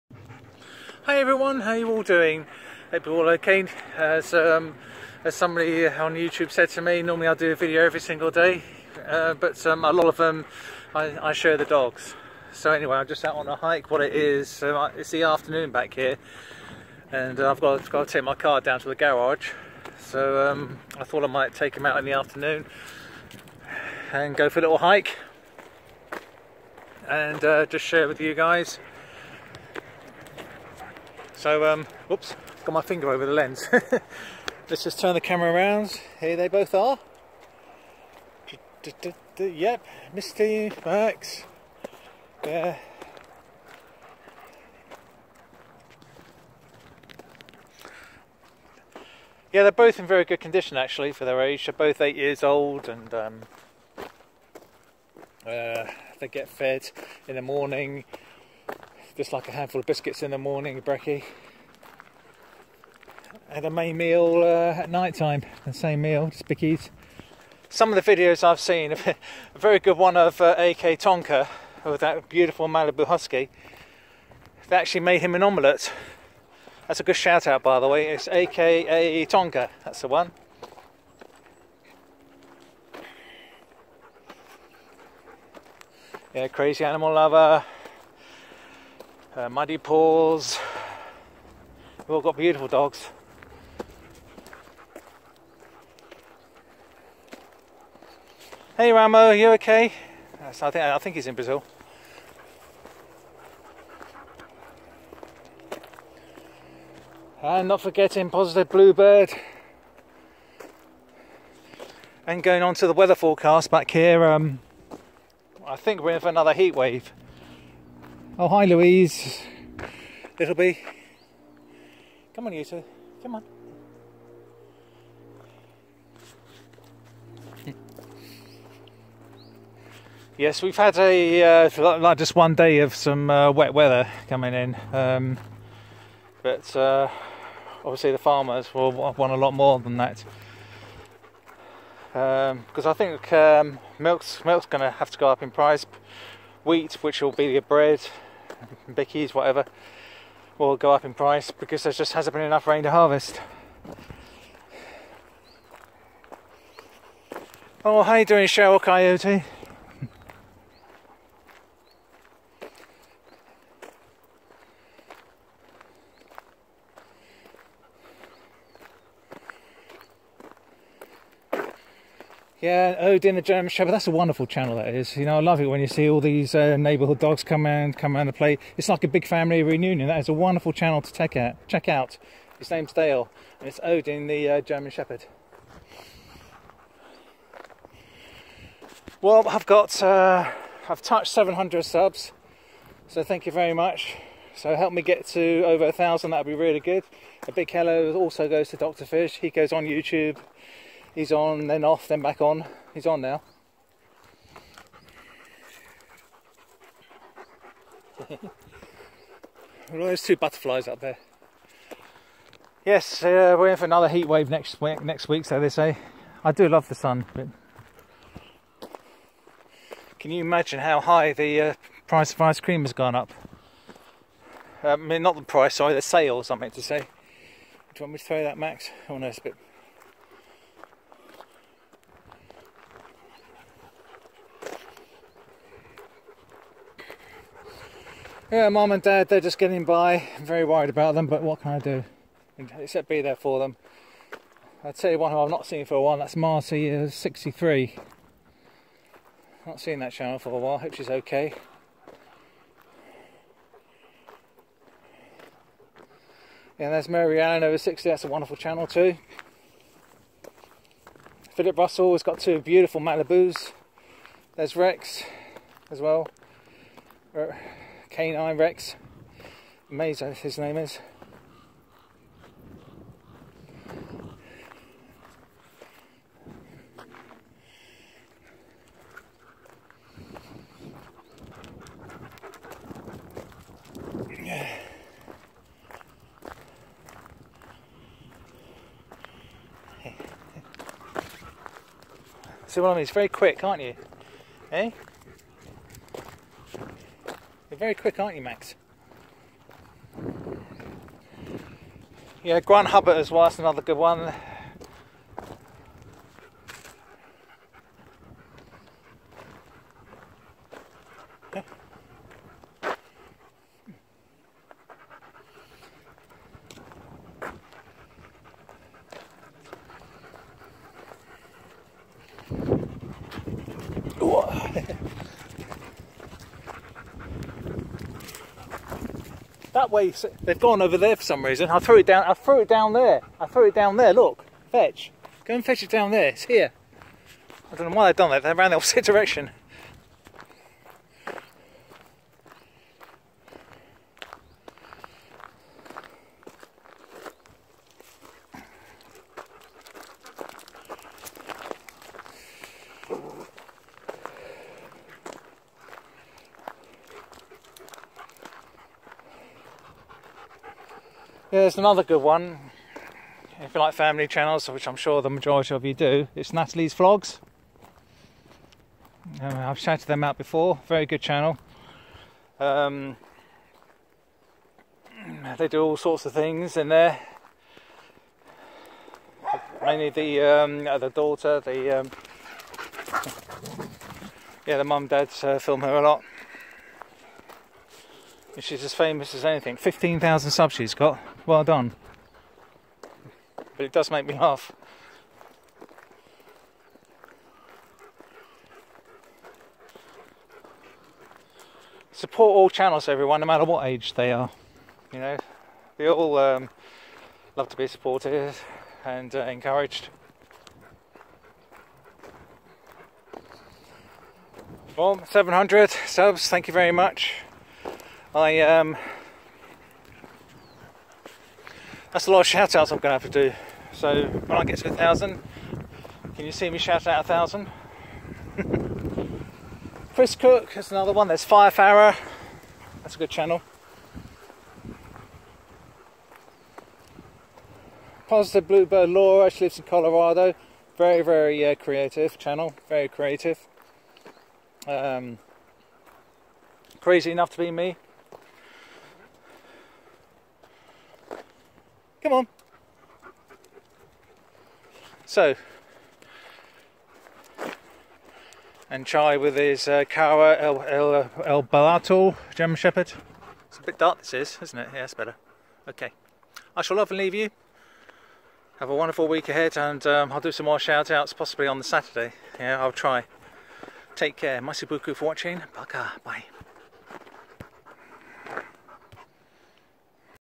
Dog walk and Shout outs.
Hi everyone, hope your all ok a bit slow in filming this week as i  have been working. The weather has been wet, its  much needed for farmers, but i like the sun like alot of people.
This is me dog walking in the woods with some great shout outs. The weather was nice, Misty and Max were fine. So enjoy the walk and shout outs, please support them. Sorry if i missed anyone, i will make up for it. Speak soon on Live stream, and other videos kind regards Carl